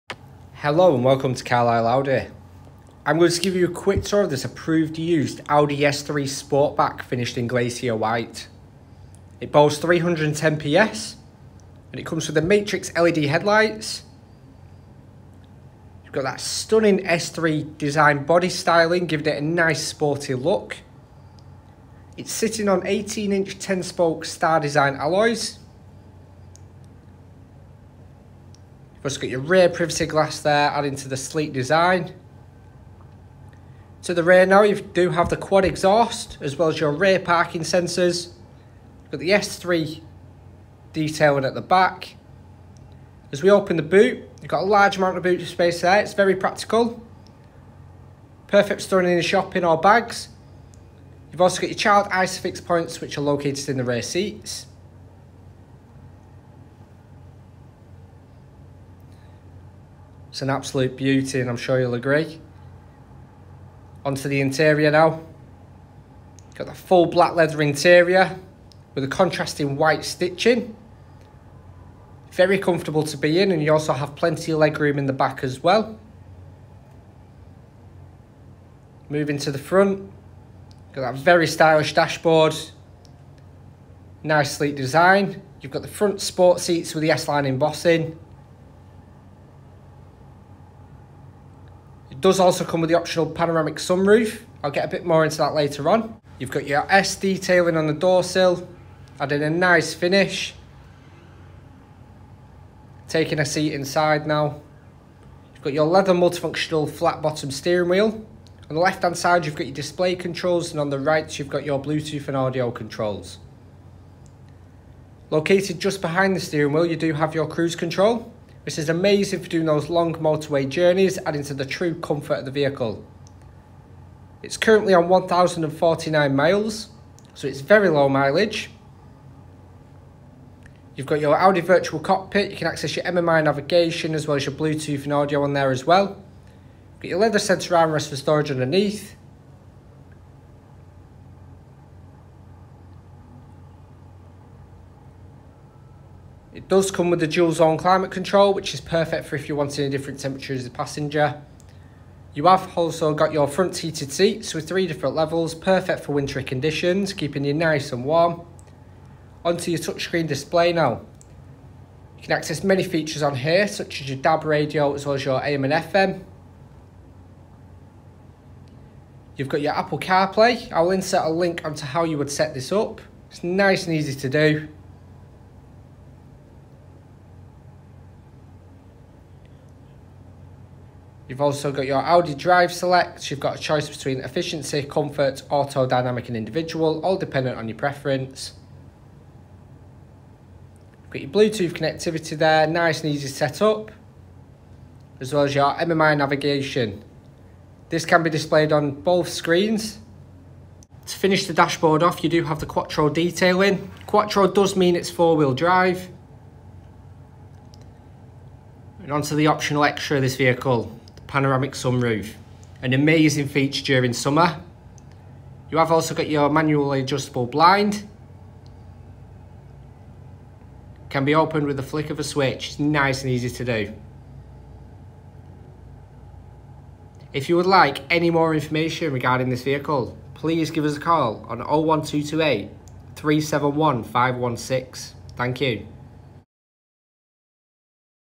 Approved used Audi S3 Sportback Quattro | Carlisle Audi
Check out this approved used Audi S3 Sportback TFSI 310 PS S tronic which is for sale at Carlisle Audi.

KW21YLG
1,049 miles
Petrol
Automatic

Features include:

Panoramic Sunroof
Rear Parking Sensors
MMI Navigation
Heated Seats
Apple CarPlay

Introduction: 0:00
Exterior: 0:09
Boot: 0:40
Interior: 1:04
How to get in touch: 3:46